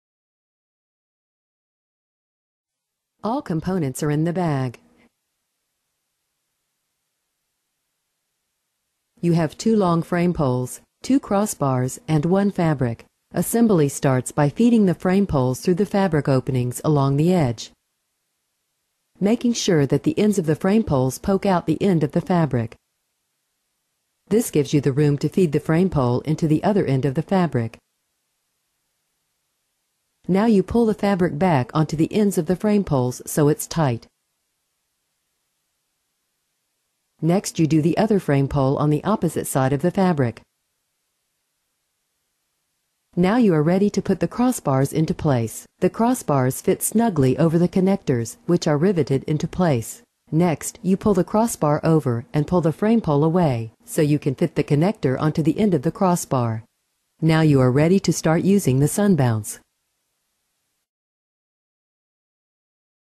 Sunbounce Mini Reflector Assembly Instructions
Assembly instructions for the California Sunbounce Mini 3x4 foot reflector.